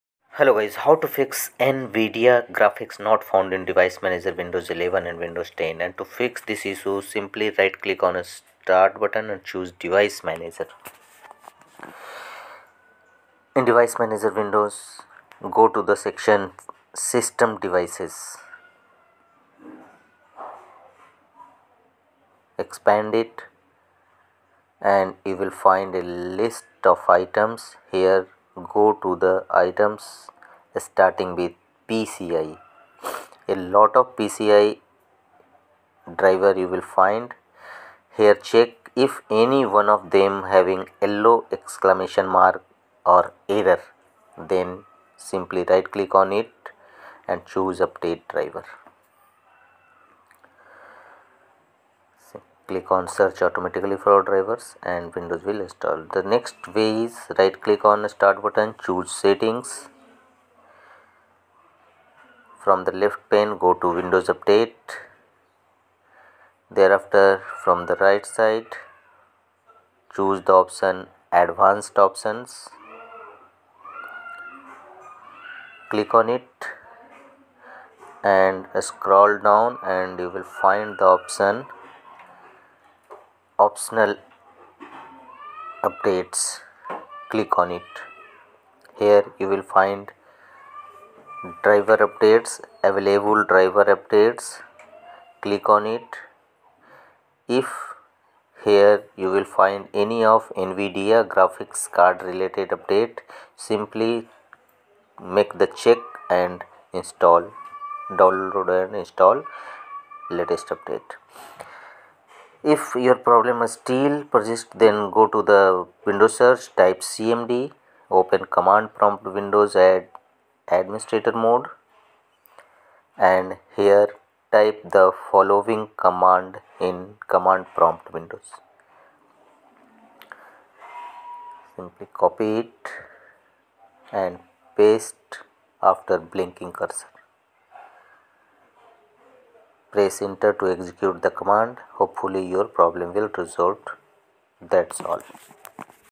NVIDIA Graphics not found in Device Manager Windows 11 and Windows 10 Fixed
How to Fix NVIDIA Graphics not found in Device Manager Windows 11 and Windows 10

Fix error